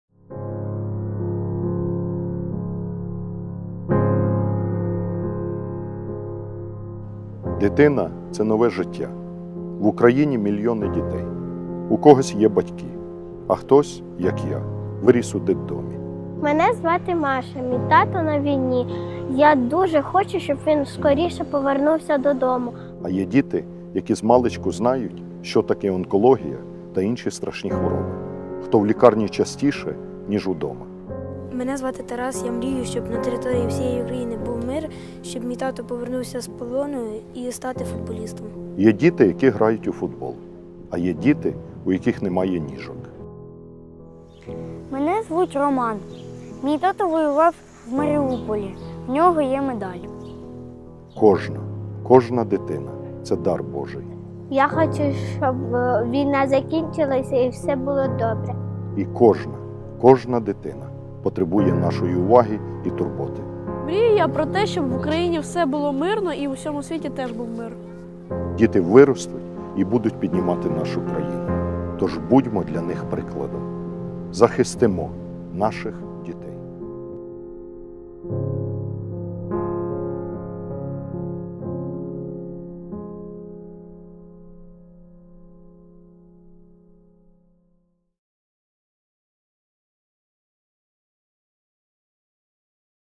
Ляшко знявся у зворушливому соціальному відеоролику до Дня захисту дітей
До Міжнародного дня захисту дітей лідер Радикальної партії Олег Ляшко записав відеоролик разом із дітьми героїв АТО, дітьми українських політв’язнів в Росії та дітьми з інвалідністю. Так політик привітав підростаюче покоління України зі святом.
«В Україні – мільйони дітей. У когось є батьки, а хтось, як я, виріс у дитбудинку. Комусь пощастило народитися здоровим, а є діти, які змалечку знають, що таке онкологія та інші страшні хвороби, хто в лікарні частіше, ніж вдома. Є діти, які грають у футбол, а є діти, у яких немає ніжок. Є діти, у яких є рідний дім. А є діти, які стали вимушеними переселенцями через війну. Кожна дитина – це дар Божий, і кожна дитина потребує нашої уваги і турботи. Діти виростуть і будуть піднімати нашу країну, тож будьмо для них прикладом. Захистимо наших дітей!», - наголосив Ляшко.